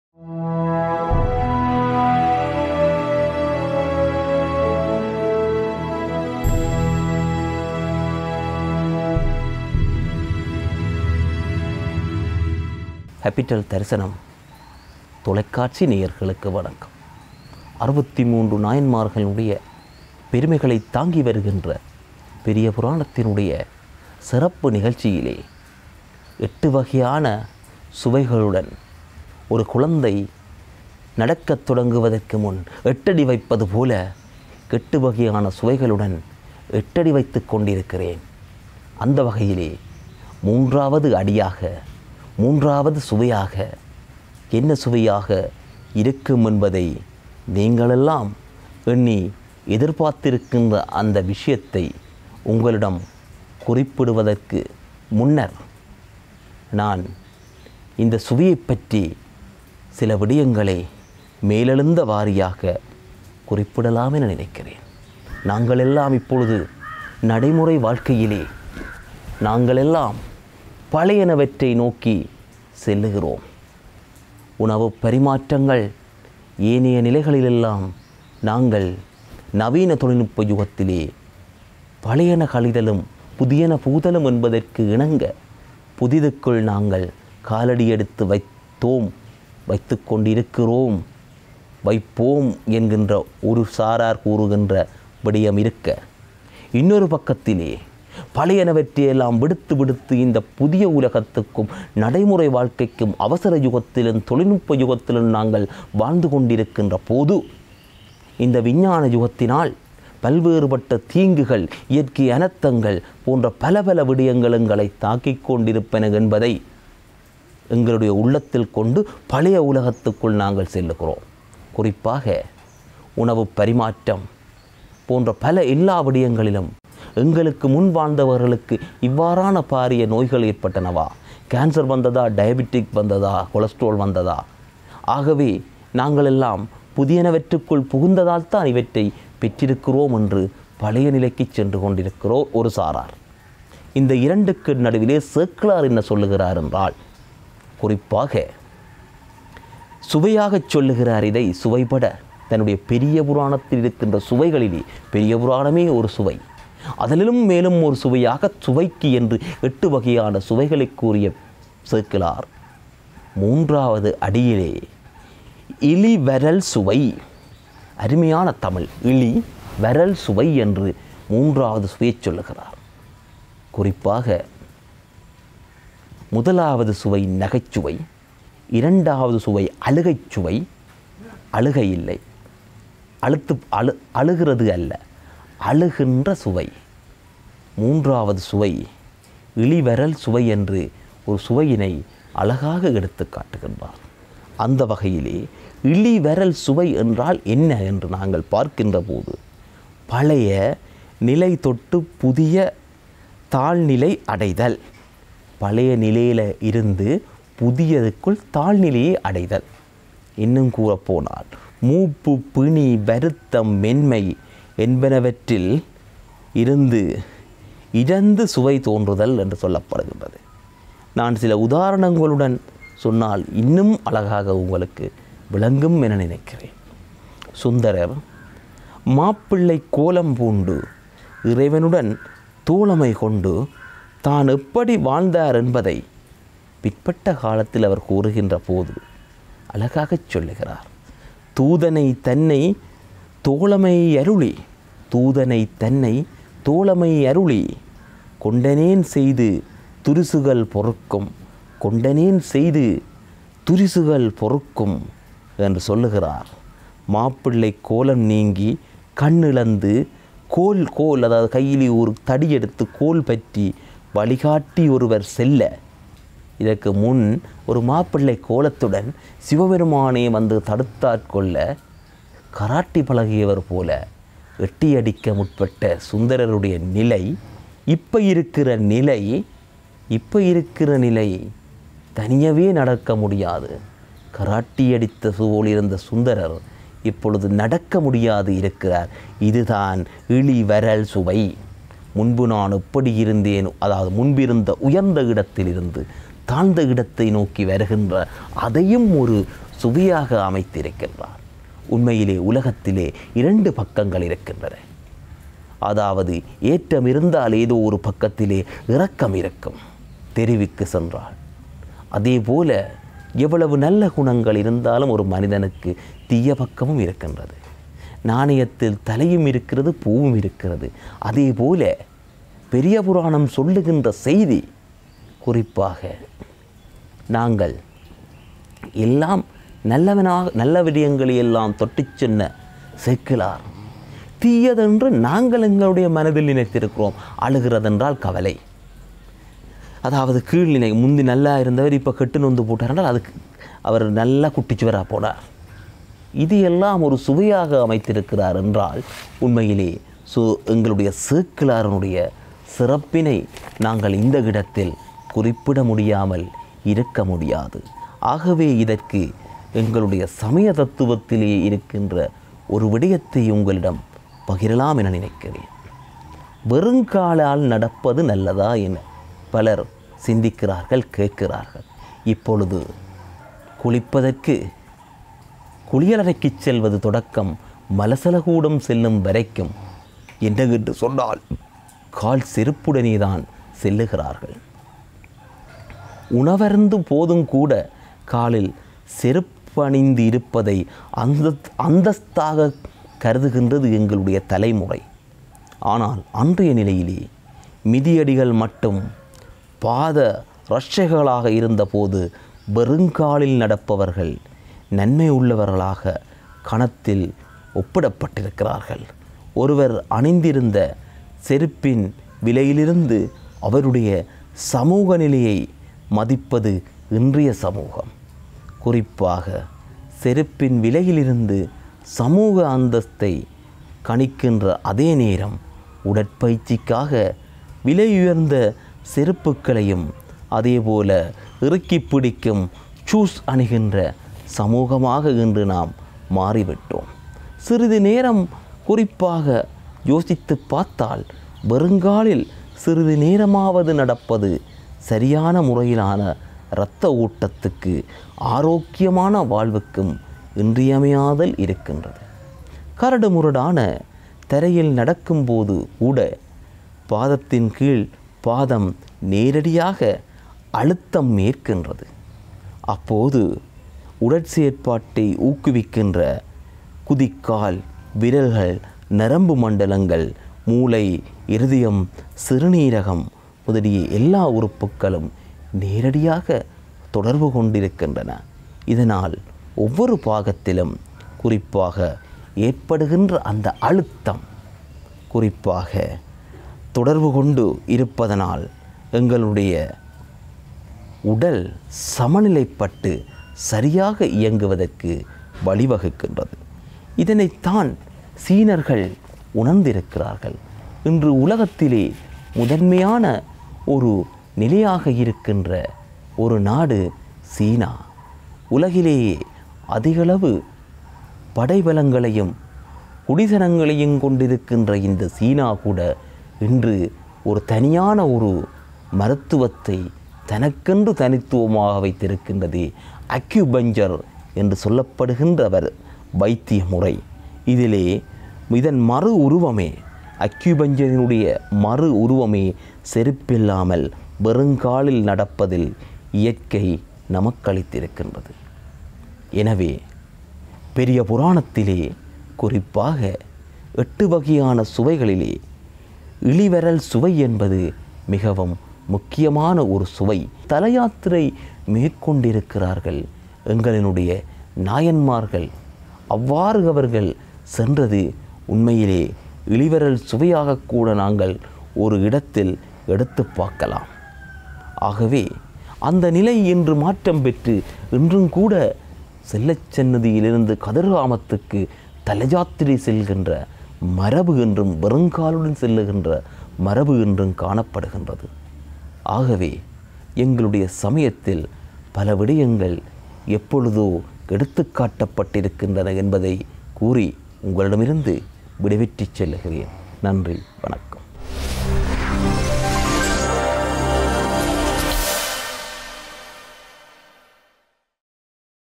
PERIYAPURANAM #167 | பெரிய புராணம் | CAPITAL THARISANAM
இன்றைய பெரிய புராணம் நிகழ்ச்சியில்………..

பேச்சாளர் மாவை.குண.கோபிராஜ்

“பெரியபுராணத்தில் சுவை”

“எட்டுவகையான சுவை”

“இழிவரல் சுவை என்றால் என்ன?”